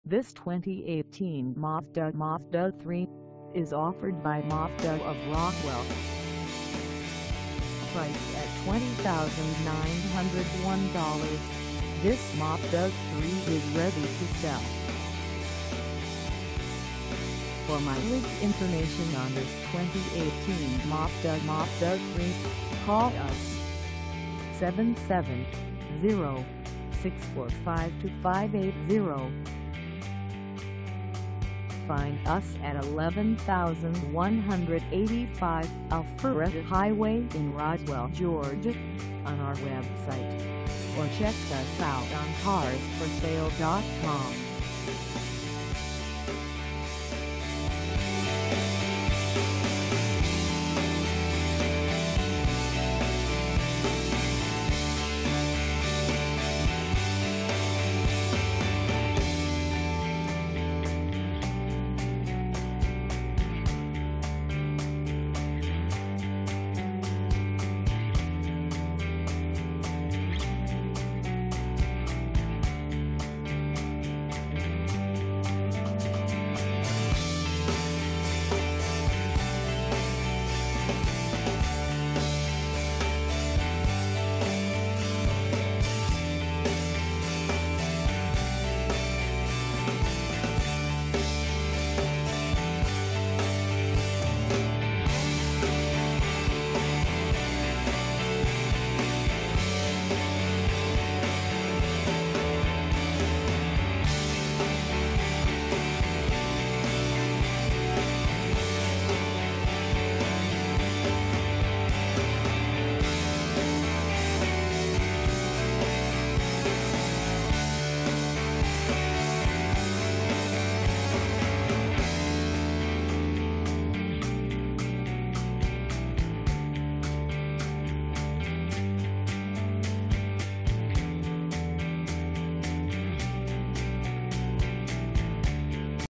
2018 Mazda MAZDA3 Touring 4dr Hatchback 6A for sale in Roswe
This 2018 Mazda MAZDA3 Touring 4dr Hatchback 6A is for sale in Roswell, GA 30076 at Mazda Of Roswell.

Engine: 2.5L I4
Transmission: Automatic 6-Speed
Color: Soul Red Metallic
Mileage: 0
Contact Mazda Of Roswell 11185 Alpharetta Hwy Roswell, GA 30076 to test drive this 2018 Mazda MAZDA3 Touring 4dr Hatchback 6A today.